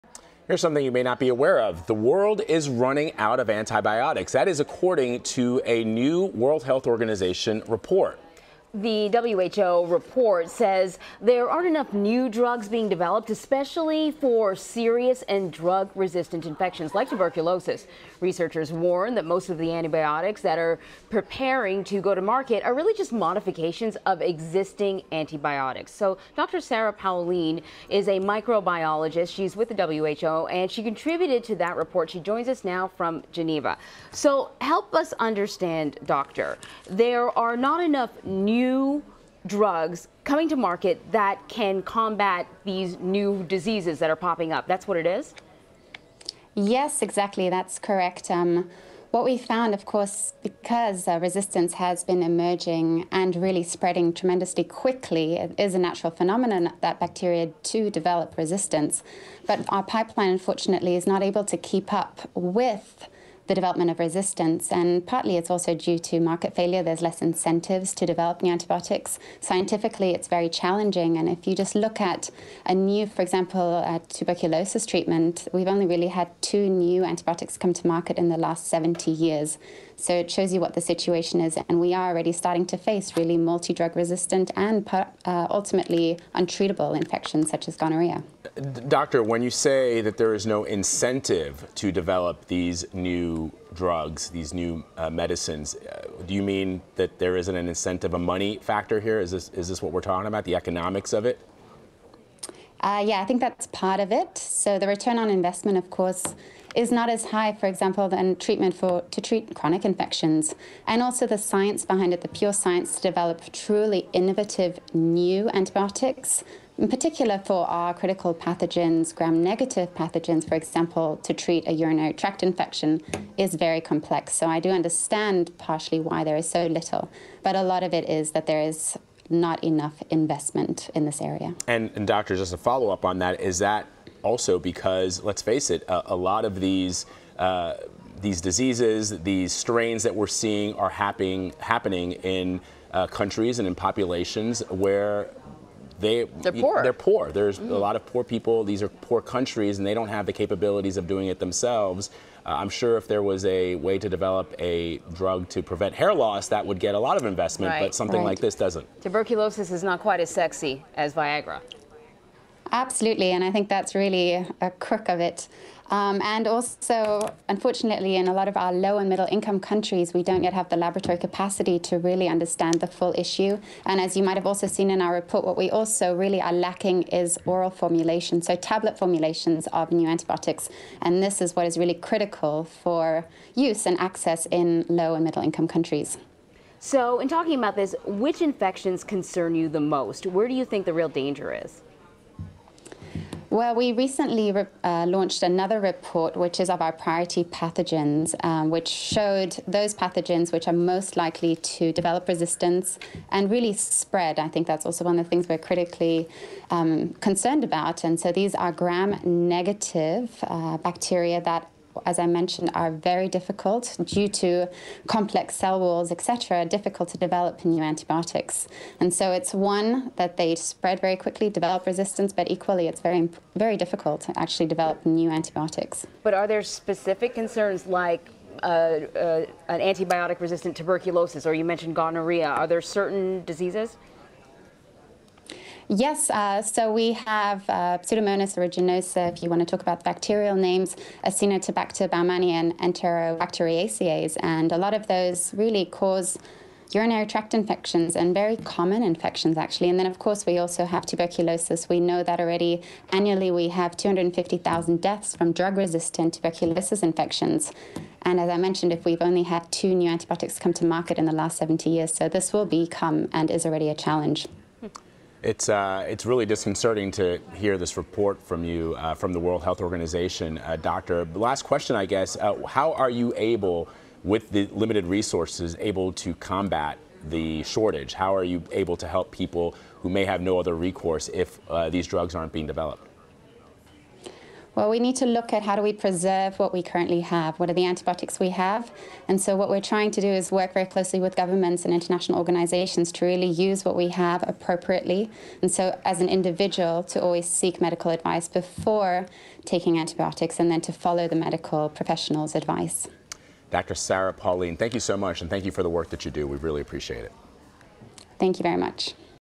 The world is running out of antibiotics
The world is running out of antibiotics, according to the World Health Organization. Dr. Sarah Paulin joined CBSN to discuss the drug-resistance crisis threatening global health.